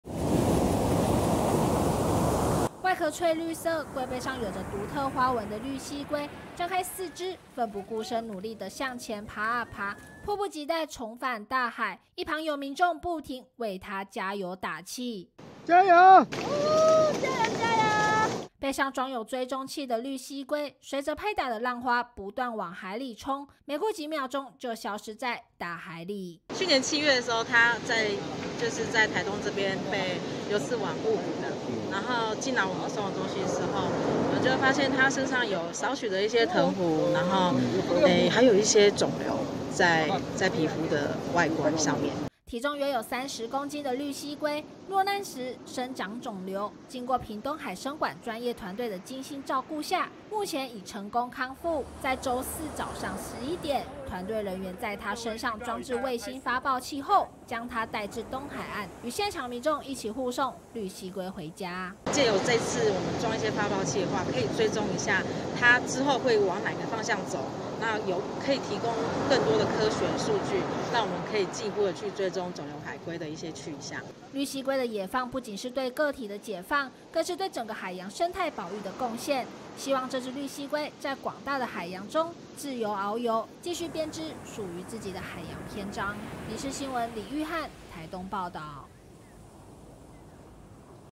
落難時被發現患有腫瘤 綠蠵龜康復後野放回歸大海－民視新聞
【民視即時新聞】地方中心／李育翰　台東報導2022年7月，一隻綠蠵龜在台東被流刺網誤傷，身上也被發現有腫瘤，經過屏東海生館獸醫團隊的照顧後，目前已康復，屏東海生館也在週四（18）早上11點，於台東杉原灣海邊，將他野放回歸大海。